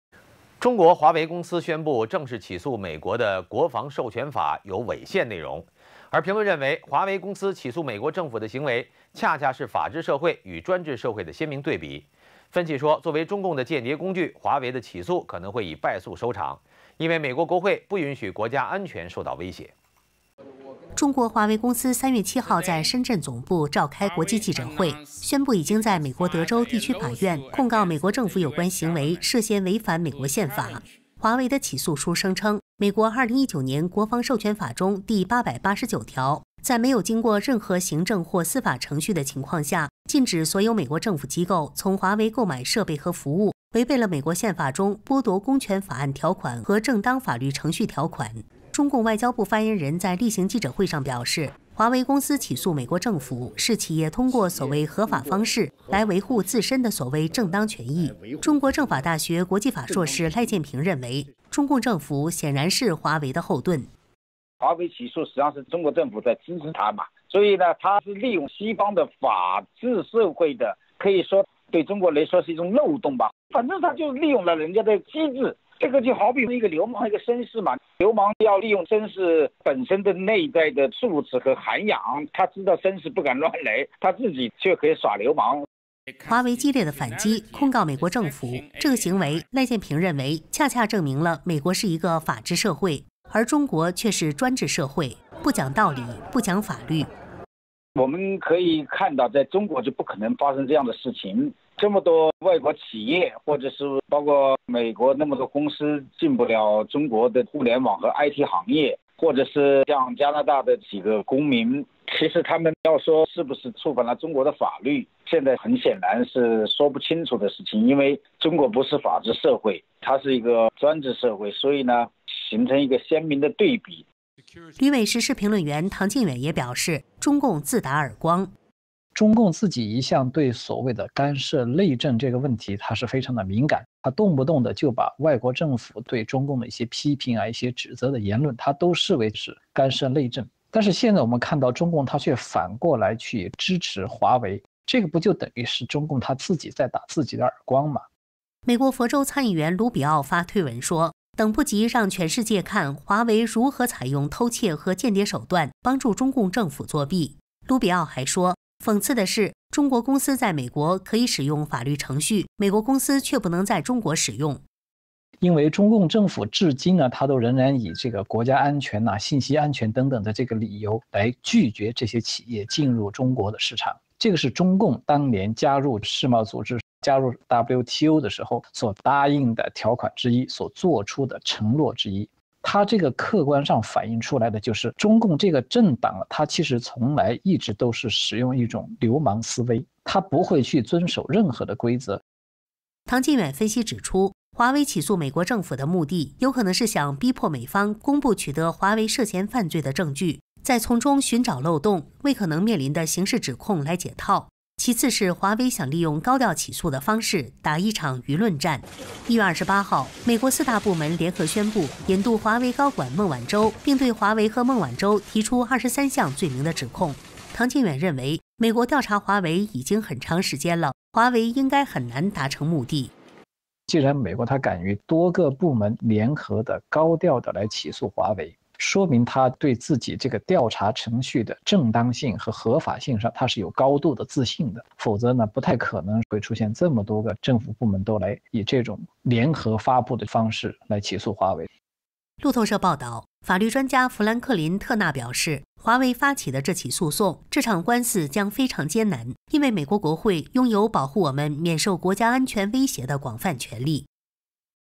华为反控美政府 专制流氓vs法治绅士【中国禁闻】
【新唐人2019年03月08日讯】中国华为公司宣布正式起诉美国的「国防授权法」有违宪内容。而评论认为，华为公司起诉美国政府的行为，恰恰是法治社会与专制社会的鲜明对比。分析说，作为中共的间谍工具，华为的起诉可能会以败诉收场，因为美国国会不允许国家安全受到威胁。

中国华为公司3月7号在深圳总部召开国际记者会，宣布已经在美国德州地区法院，控告美国政府有关行为，涉嫌违反美国宪法。

华为的起诉书声称，美国《2019年国防授权法》中第889条：在没有经过任何行政或司法程序的情况下，禁止所有美国政府机构从华为购买设备和服务，这违背了美国宪法中剥夺公权法案条款、和正当法律程序条款。

中共外交部发言人在例行记者会上表示，华为公司起诉美国政府，是企业通过所谓合法方式，来维护自身的所谓正当权益。中国政法大学国际法硕士赖建平认为，中共政府显然是华为的后盾。

中国政法大学国际法硕士赖建平：「华为起诉（美国），实际上是中共政府在支持它，所以它是利用西方的法治社会的，可以说，对中国来说是一种漏洞，反正它就是利用人家的机制。这好比是一个流氓和绅士，流氓要绅士本身的内在素质和涵养，它自己知道绅士不敢乱来，它自己却可以耍流氓。」

华为激烈的反击，控告美国政府，这个行为，赖建平认为，恰恰证明了美国是一个法治社会，而中国却是专制社会，是不讲道理，不讲法律。

赖建平：「我们可以看到在中国是不可能发生这样的事情。这么多外国企业或者是包括美国那么多公司，进不了中国的互联网和IT行业。或者是像加拿大的几个公民，其实要说他们是不是触犯了中国的法律，现在很显然是说不清楚的事情，因为中国不是法治社会，它是一个专制社会，所以形成一个鲜明的对比。」

旅美时事评论员唐靖远也表示：中共自打耳光。

旅美时事评论员唐靖远：「中共自己一向对所谓的干涉内政这个问题，它是非常的敏感，它动不动的就把外国政府对中共的一些批评、一些职责的言论，它都视为这是干涉内政。但是现在我们看到中共它却反过来去支持华为，这个不就等于是中共它自己在打自己的耳光吗？」

美国佛州参议员卢比奥发推文说，等不及让全世界看「华为如何采用偷窃和间谍手段，帮助中共政府作弊」了。卢比奥还说，「讽刺的是，中国公司在美国可以使用法律程序，美国公司却不能在中国使用（法律程序）。」

唐靖远：「因为中共政府至今它都仍然以这个国家安全、信息安全等等的这个理由来拒绝这些企业进入中国市场，这个是中共当年加入世贸组织，加入WTO的时候，所答应的条款之一，所作出的承诺之一。它这个客观上反映出来的，就是中共这个政党它其实从来一直都是使用一种流氓思维，它不会去遵守任何的规则。」

唐靖远分析指出，华为起诉美国政府的目地，有可能是想逼迫美方公布取得华为涉嫌犯罪的证据，再从中寻找漏洞为可能面临的刑事指控来解套。其次，是华为想利用高调起诉的方式，打一场舆论战。

1月28号，美国四大部门联合宣布：引渡华为高管孟晚舟，并对华为和孟晚舟提出23项罪名的指控。唐靖远认为，美国调查华为已经很长时间了，华为应该很难达成目地。

唐靖远：「既然美国它敢于多个部门联合的、高调的来起诉华为，说明它对自己这个调查程序的正当性和合法性上，它是有高度的自信，否则不太可能会出现这么多个政府部门，都来以这种联合发布的方式来起诉华为。」

路透社报导，法律专家富兰克林‧特纳（Franklin Turner）表示，华为发起的这起诉讼，「这场官司将非常艰难，因为美国国会拥有保护我们免受国家安全威胁的广泛权力。」

采访/常春 编辑/黄亿美 后制/钟元